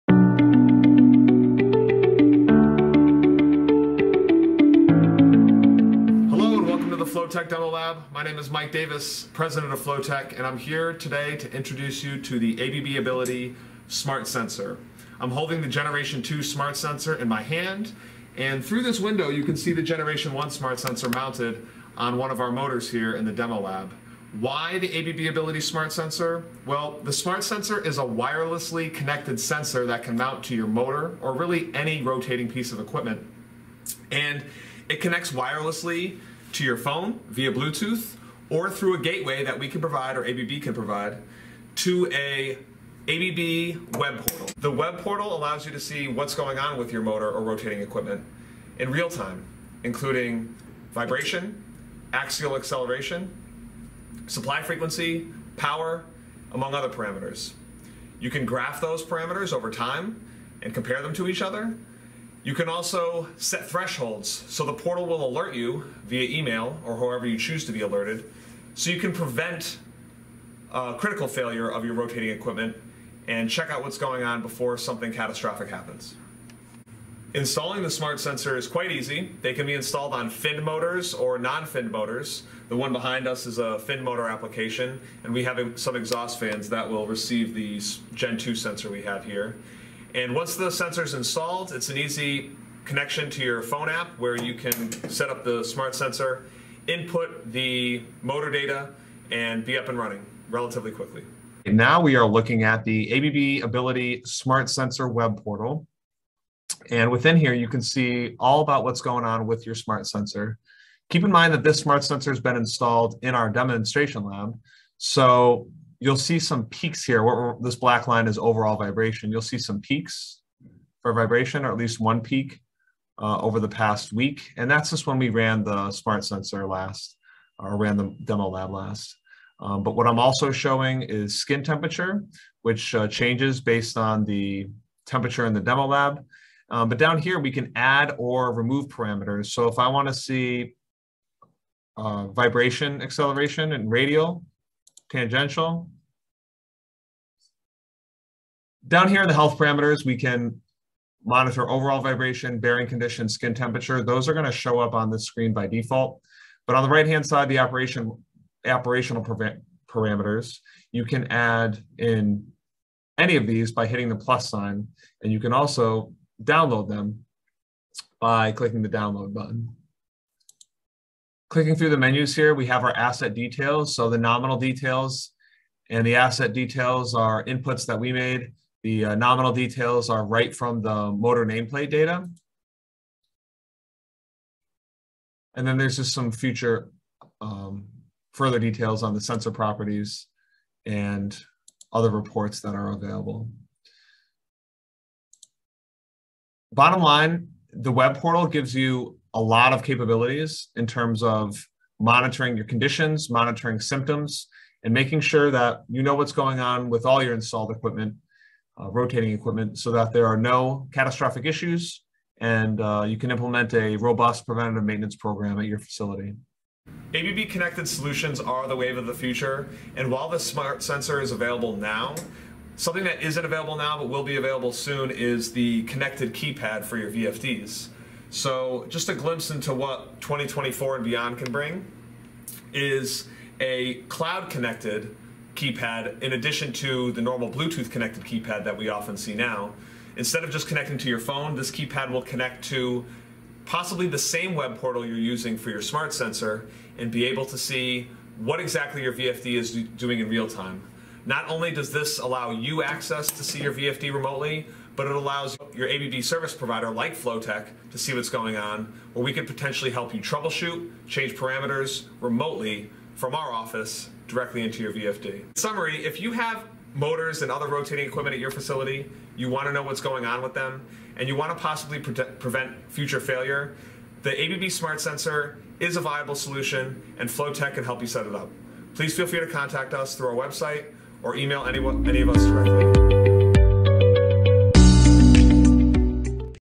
ABB Ability Smart Sensor
The Smart Sensor converts traditional motors into smart, wirelessly connected devices. It enables users to monitor the health of their motors and to plan maintenance in advance.

Easily attached to an existing motor either mechanically or via epoxy, the Smart Sensor can wirelessly connect via Bluetooth to your cell phone or to a wireless gateway. The data is uploaded to an ABB cloud service where Flow Tech and/or the end user can monitor their motor, fan, pump, bearing for vibration, skin temperature, speed, etc. and set alarms for the health of their equipment.